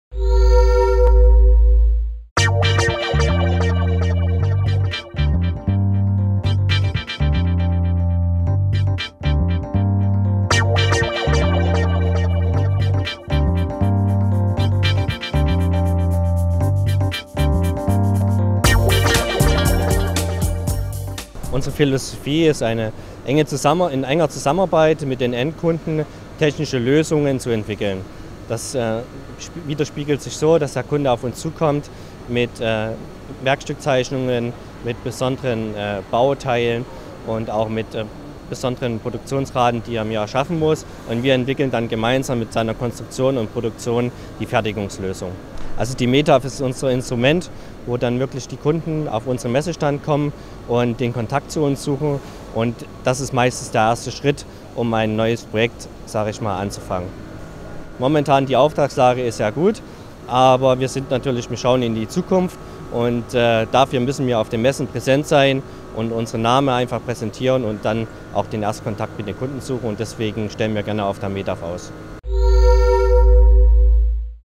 METAV 2014: Danobat Group arbeitet eng mit seinen Kunden zusammen
Toni Neugebauer ist Area Sales Manager und spricht über die Philosophie von Danobat, die eine enge Zusammenarbeit mit den Kunden vorsieht und über die momentane Auftragslage.